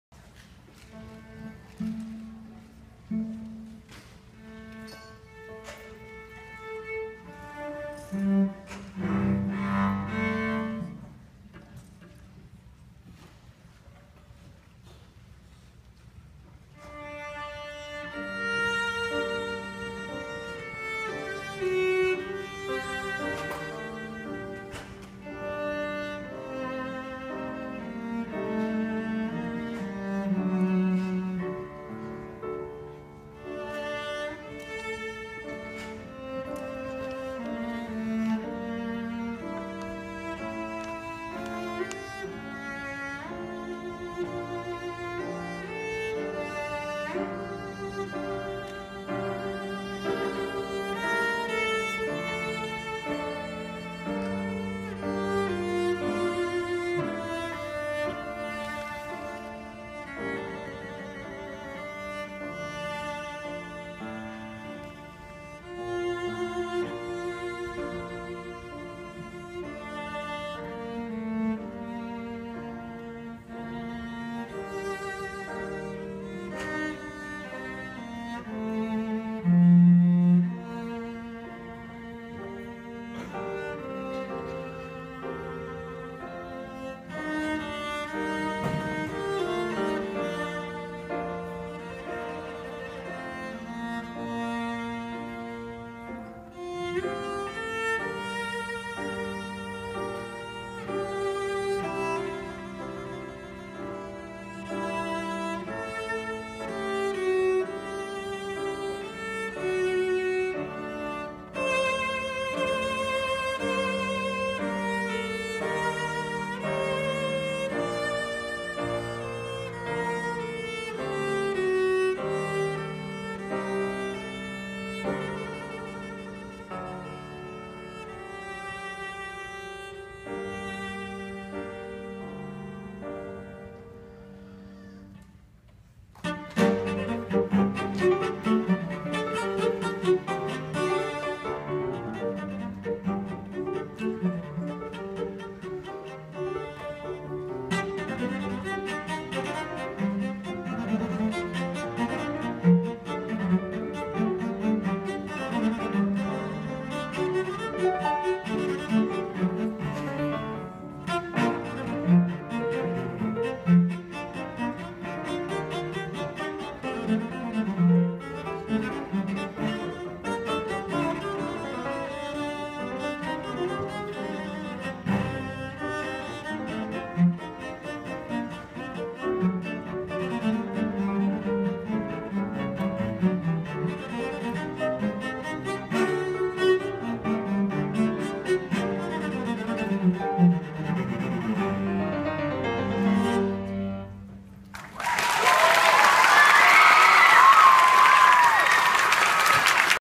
Hannah Kim amazing cello solo
This is Hannah I kinda know her she's in orchestra at my school she's an amazing player and a good friend even if I don't know her that well